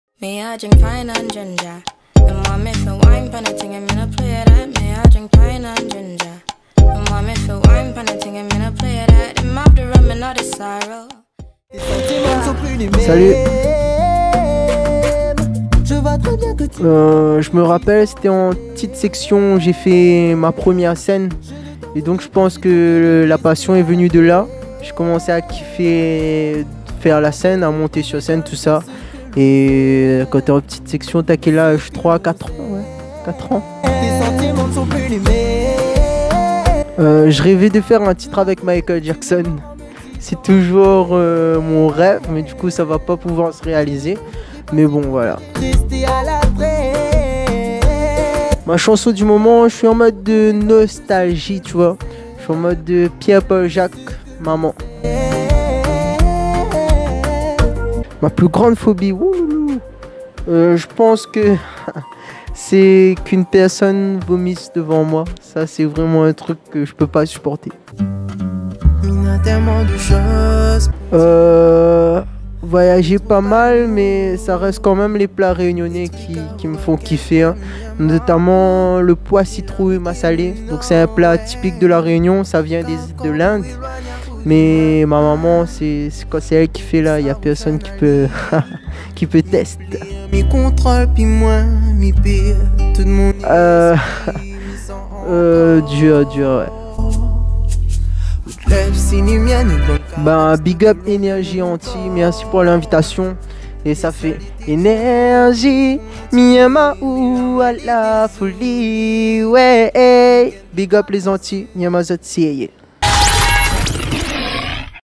MikL dans labox NRJ Antilles
LaBox Le lover réunionnais MikL nous a accordé un entretien lors de son passage en Martinique.